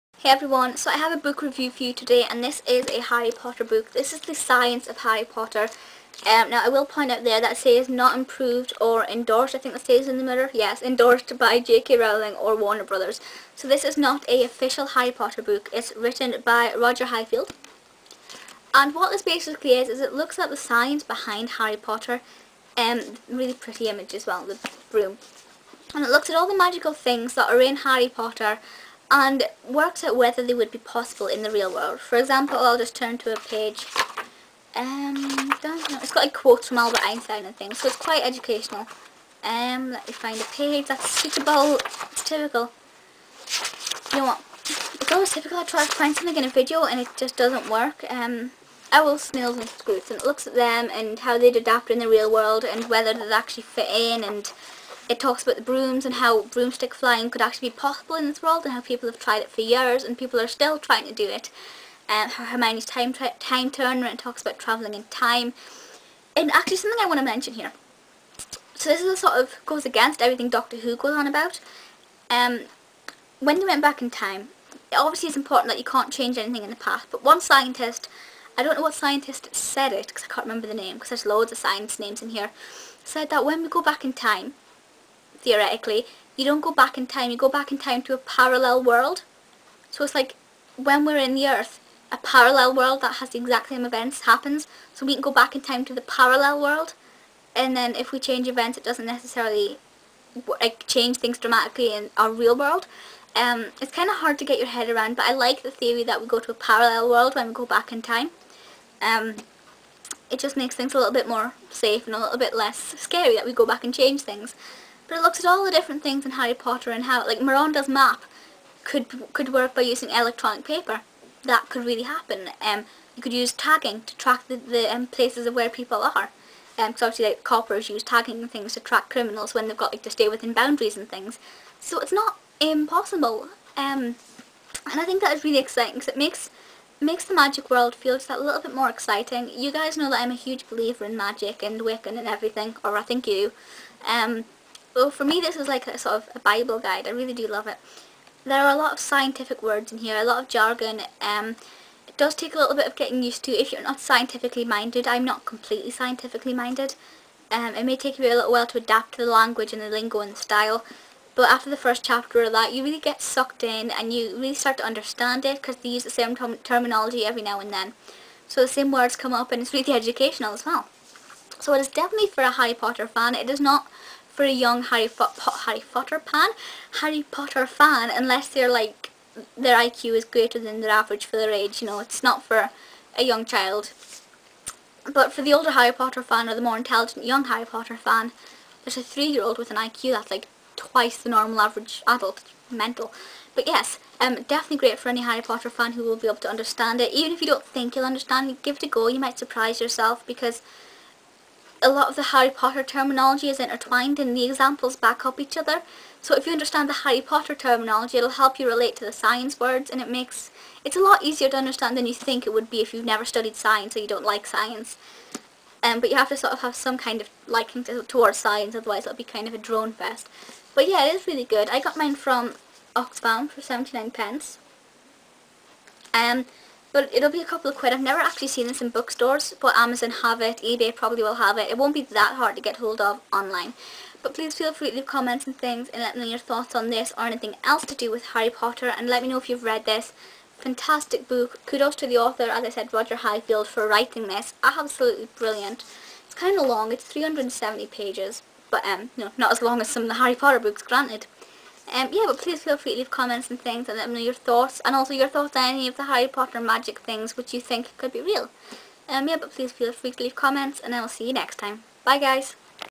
The Science of Harry Potter by Roger Highfield | Review
MY BOOKS What's your review of the Science of Harry Potter Roger Highfield book? [OPEN FOR LINKS AND INFO!] HarryPotter magic science Do you love Harry Potter books? This is great for learning about the physics of Harry Potter, science experiments, and debating whether or not Harry Potter science fiction? I'd love to know your review and reaction to this! Scroll down for more information!

Amy McLean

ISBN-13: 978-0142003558